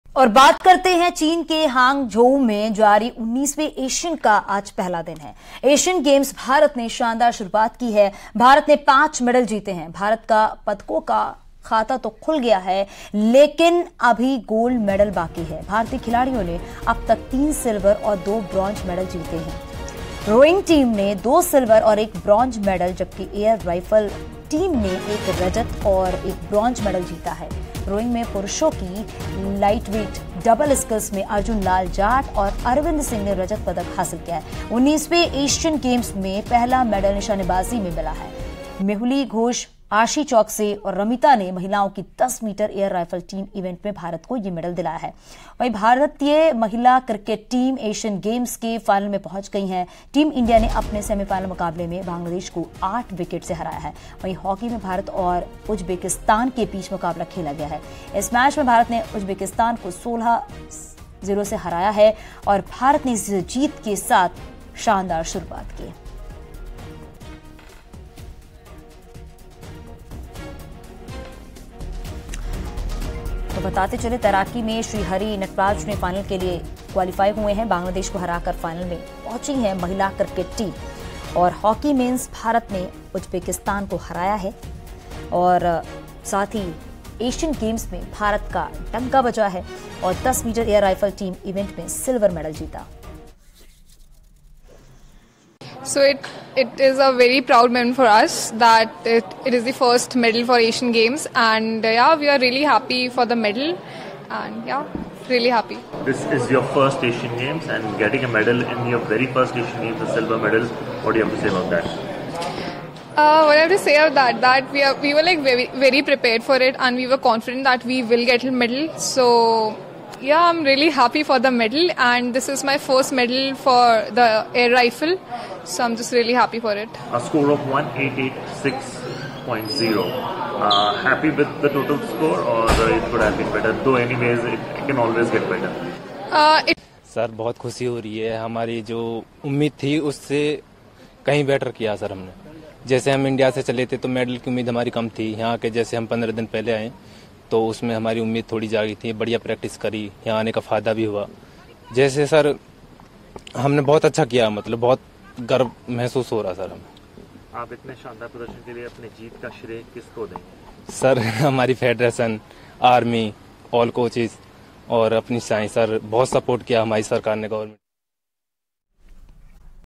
Asain Games 2023 में भारत ने दिखाया जलवा | Breaking News | Team India | Medal | Trending News
एशियन गेम्स में भारत के लिए मेडल का खाता खुल चुका है. निशानेबाजी और रोइंग और शूटिंग में भारत के खिलाड़ियों ने मेडल जीतने में सफलता हासिल कर ली है.